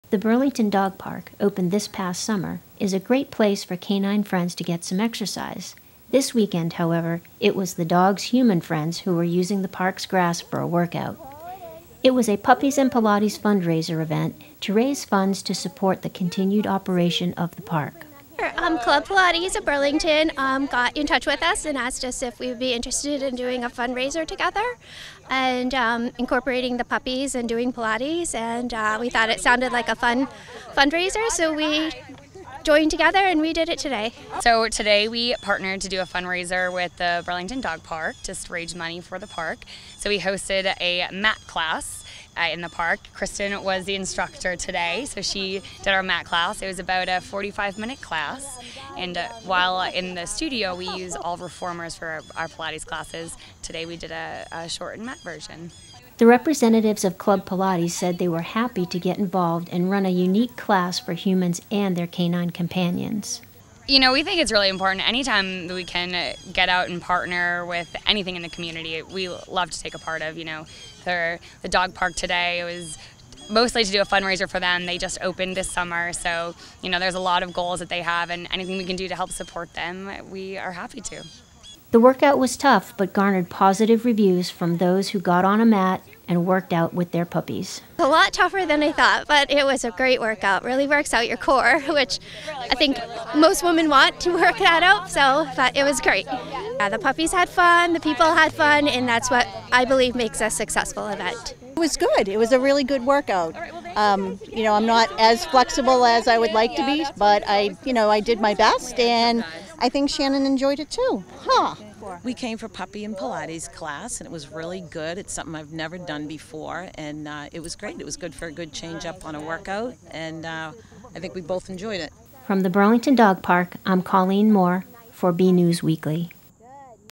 Burlington Dog Park Puppies & Pilates Fundraiser
It wasn't just pups getting some exercise at the Burlington Dog Park this past Saturday as the Friends of the Burlington Dog Park and Club Pilates teamed up for a fundraiser that combined Pilates and puppies